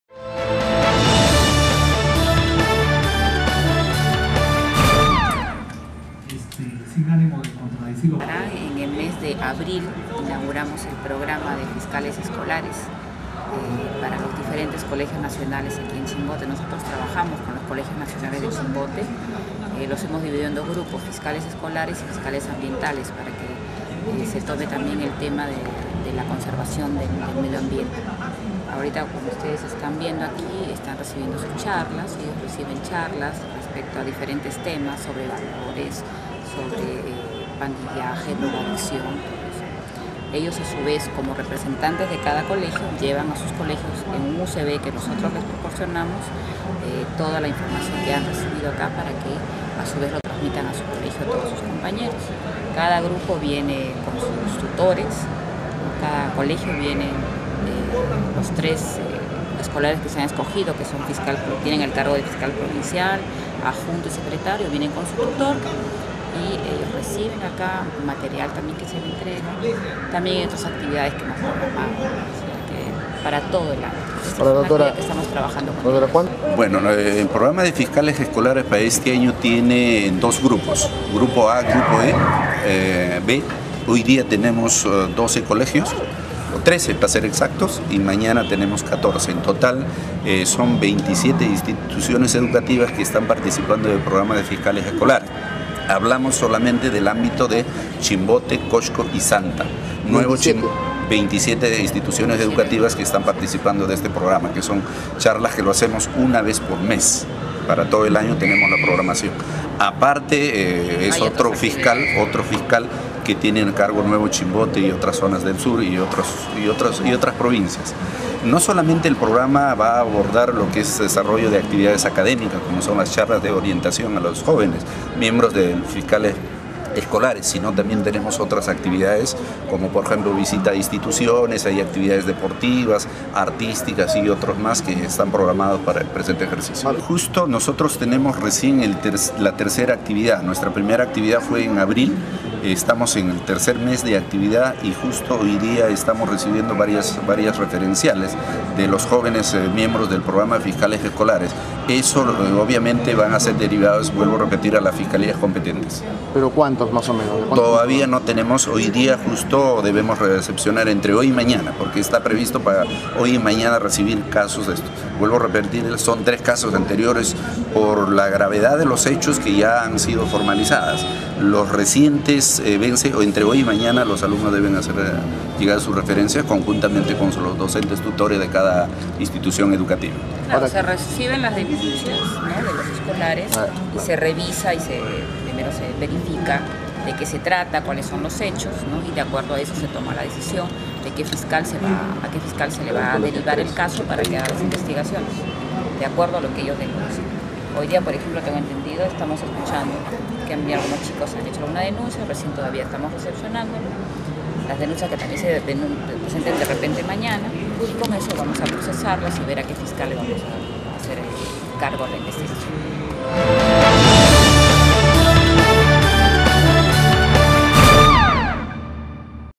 EN ABRIL SE INAUGURÓ EL PROGRAMA DE FISCALES ESCOLARES PARA LOS DIFERENTES COLEGIOS NACIONALES
En el mes de abril se inauguró el programa de fiscales escolares para los diferentes colegios nacionales de Chimbote. Hasta el momento, se han presentado 27 centros educativos a dichas charlas, las cuales ayudarán a los alumnos a poder denunciar cualquier caso de abuso o algún otro trato que perjudique a los alumnos de los centros educativos.
3 casos en dos centros educativos se han presentado por parte de estos fiscales escolares, quienes ya denunciaron ante los entes correspondientes, así lo manifestaron la fiscal superior Carmen Trujillo Marcelo y el responsable de fiscales escolares 2011 el fiscal Javier López Álvarez en las instalaciones del Ministerio Público.